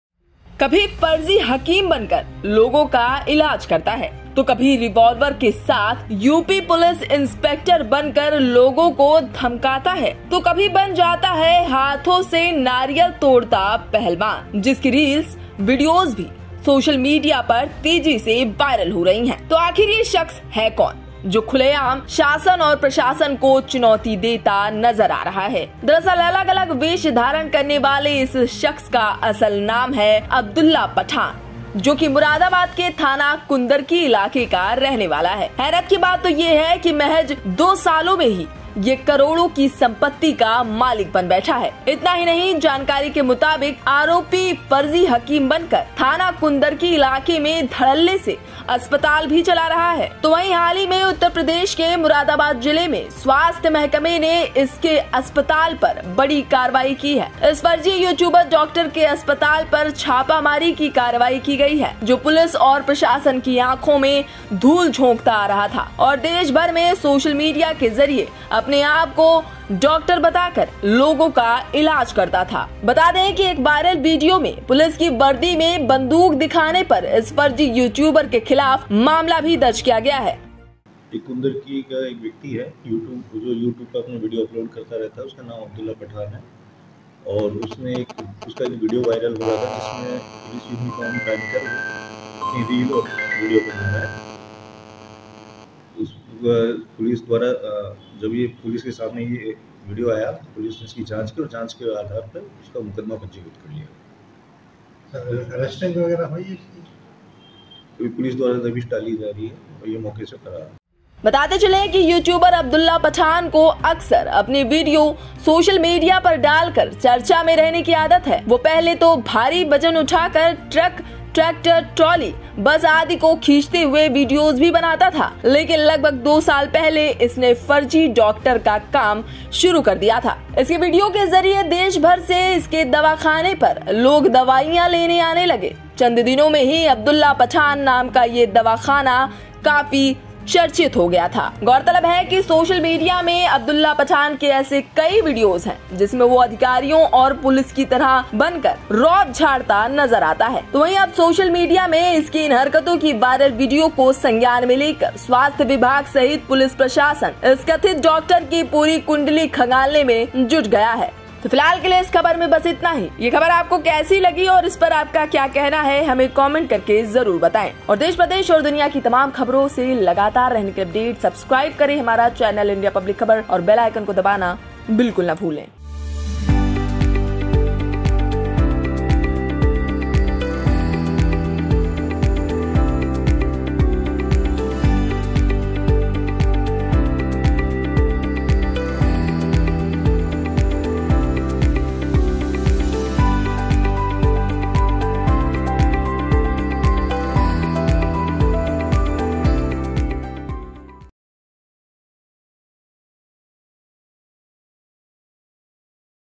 Police Uniform में Reels बनाने वाले Youtuber के खिलाफ मामला दर्ज
उत्तर प्रदेश के मुरादाबाद जिले में स्वास्थ्य विभाग ने इस पर अस्पताल पर बड़ी कार्रवाई की है इस फर्जी यूट्यूबर डॉक्टर के अस्पताल पर छापामार कार्यवाही की गई है, जो पुलिस और प्रशासन की आंखों में धूल झोंकता आ रहा था और देश भर में सोशल मीडिया के जरिए अपने आपको डॉक्टर बता कर लोगों का ईलाज करता था

In the eastern districts of Uttar Pradesh, the Health Department has taken major action against this hospital. The hospital of this fake YouTuber doctor has been accused of dishonesty, who was throwing dust in the eyes of the police and administration and was spreading his message across the country through social media. tell the doctor about people's treatment

About Channel